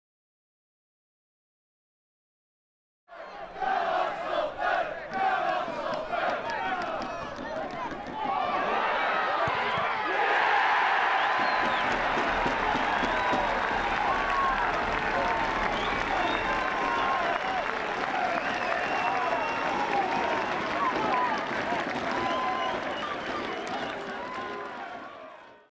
October Goal of the Month - James Poole vs Boston United
James Poole's goal against Boston United won October's Goal of the Month competition with 38.2% of the votes!